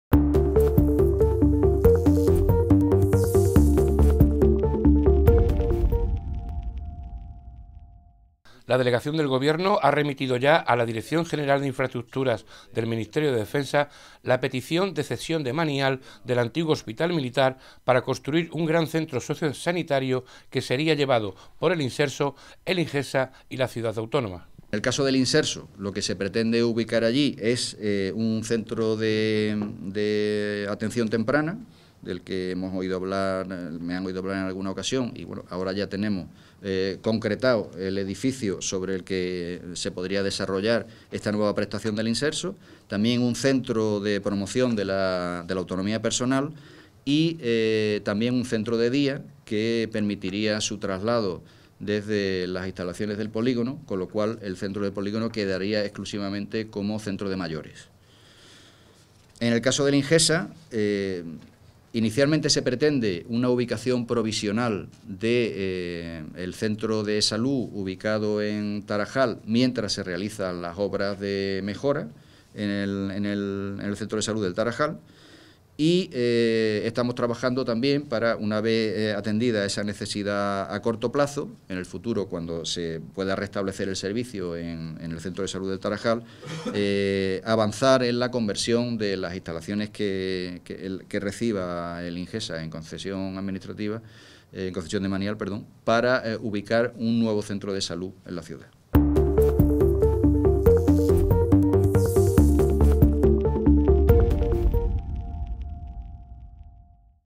Delegación ha solicitado ya a Defensa la cesión del antiguo Hospital Militar
La Delegación del Gobierno ya ha solicitado al Ministerio de Defensa la cesión demanial del antiguo Hospital Militar para la construcción de un gran centro sociosanitario que estaría gestionado por el Imserso, Ingesa y la Ciudad Autónoma.
La razón de que haya sido la propia Delegación la que ha encabezado todas estas negociaciones se debe a una propia recomendación de la Dirección General de Infraestructuras del Ministerio de Defensa con la intención de acelerar más el proyecto